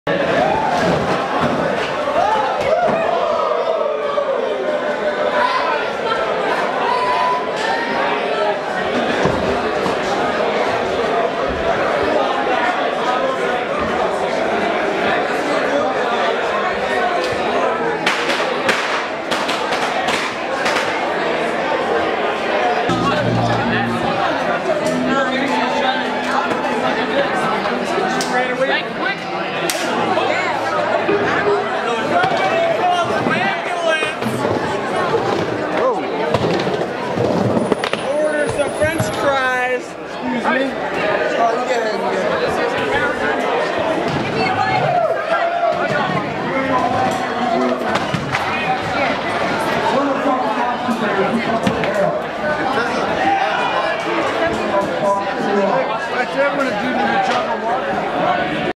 Girl takes skateboard in the head
From Bowl Bash 17 at Skatopia in Rutland, OH